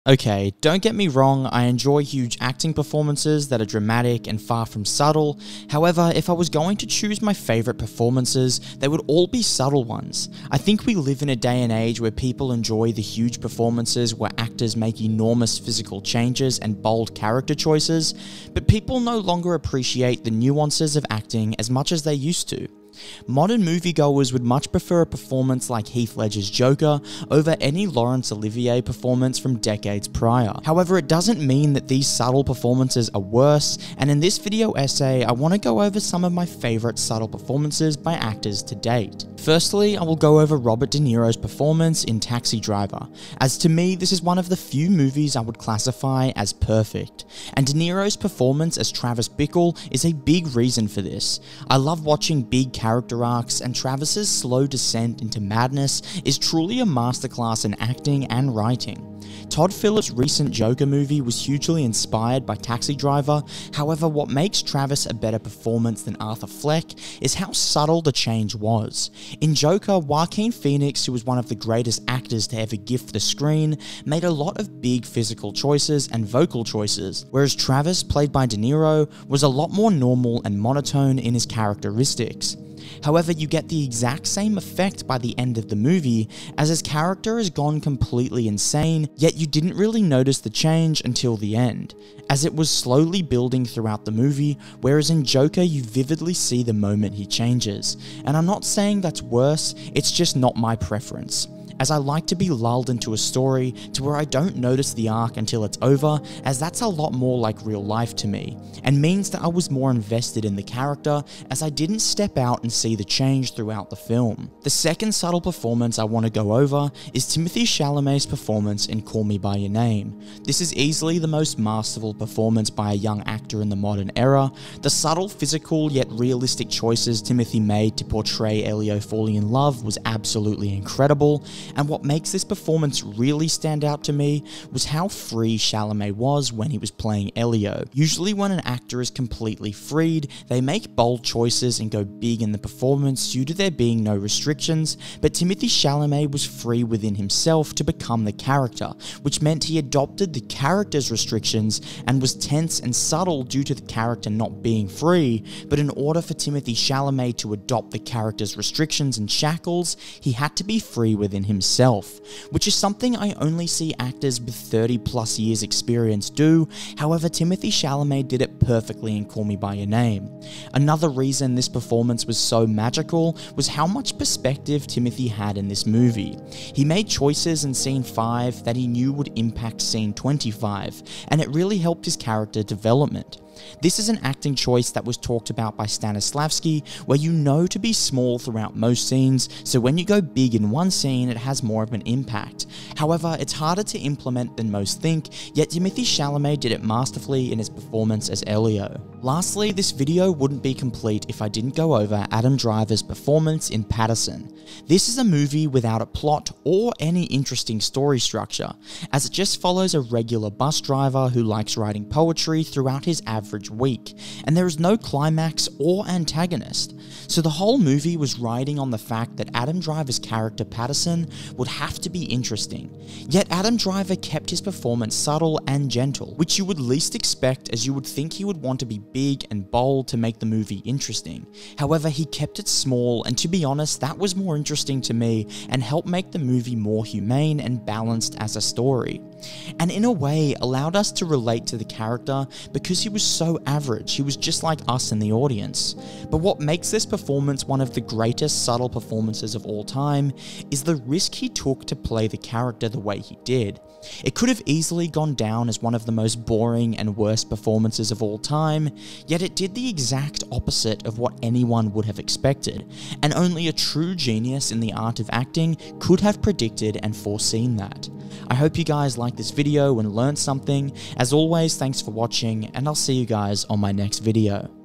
The Art Of Subtle Performance (Video Essay)
recently it seems like a lot of the most talked about movies are the big ones with actors making bold choices and playing grand characters. However in my opinion some of the best performances of all time are quite subtle and tend to go unnoticed a lot of the time. In this video essay I break down my favourite subtle performances from actors like Timothee Chalamet, Robert De Niro and Adam Driver. As well as go over what makes these subtle performances so powerful.